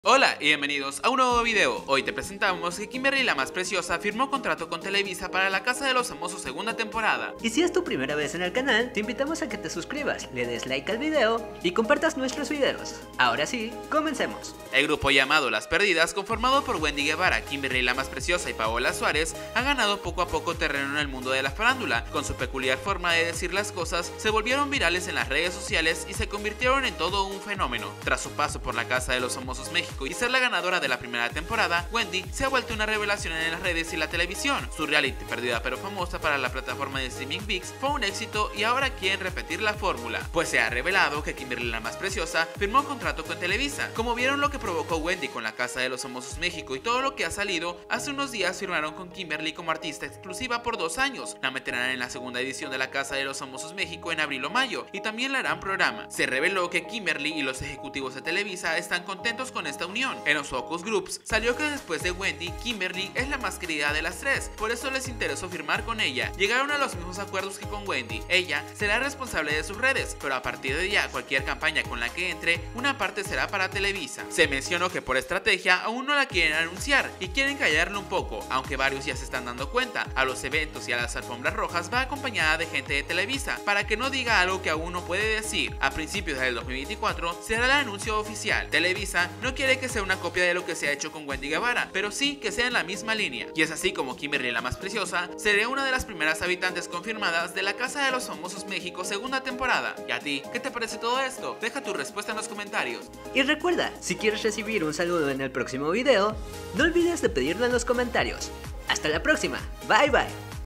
Kimberly ‘La Más Preciosa’ Confirmada Para ‘LA CASA DE LOS FAMOSOS MÉXICO’ 2 Firma Con Televisa
En este video te mostramos qué KimberlyLaMásPreciosa firma contrato con Televisa y se convierte en la primera confirmada para la segunda temporada de LaCasaDeLosFamososMx